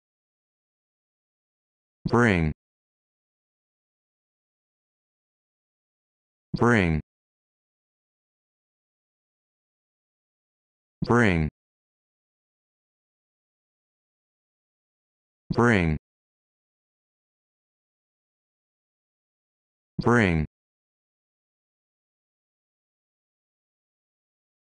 How to Pronounce Bring
This video shows you how to pronounce Bring